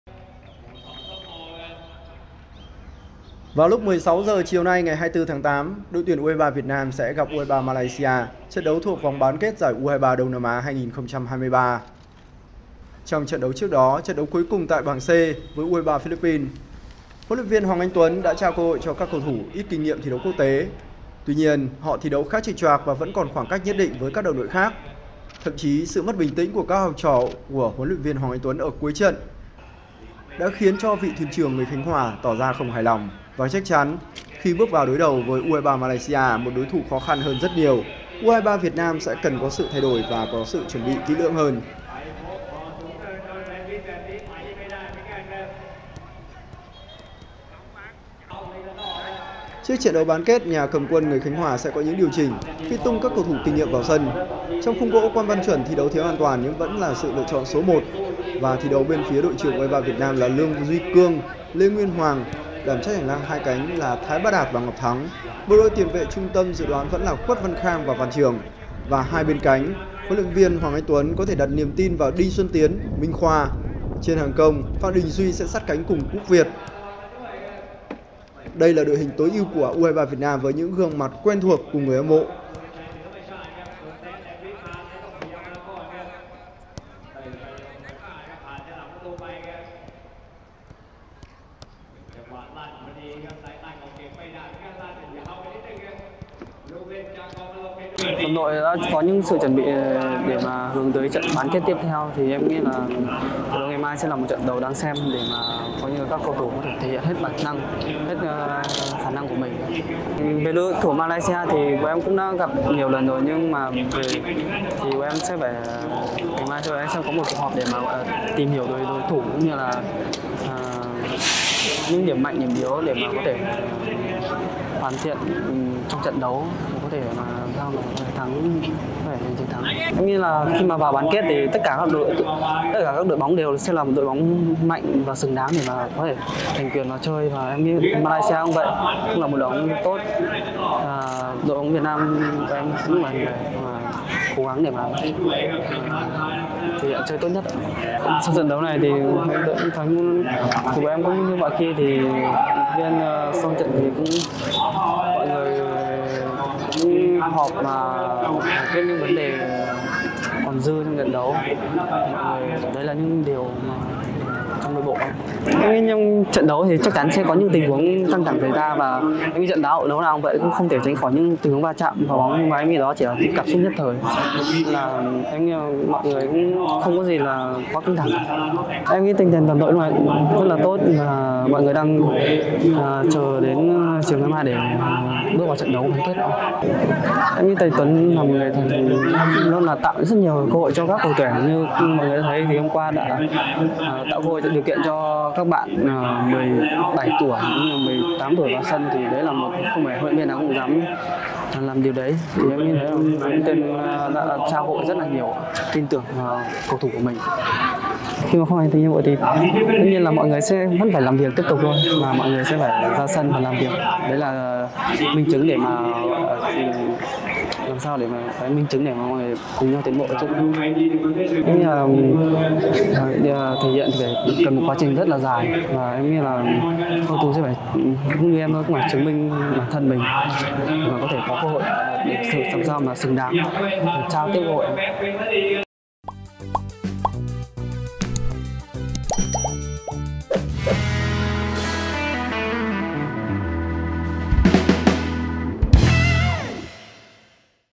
U23 Việt Nam tập luyện chuẩn bị quyết đấu U23 Malaysia | Giải Vô địch Bóng đá U23 Đông Nam Á
Trận đấu giữa U23 Việt Nam vs u23 Malaysia diễn ra lúc 16h00 ngày 24/8 trên sân Rayong Provicial (Thái Lan). Bước vào trận bán kết, nhà cầm quân gốc Khánh Hòa sẽ có những điều chỉnh khi tung các cầu thủ kinh nghiệm vào sân.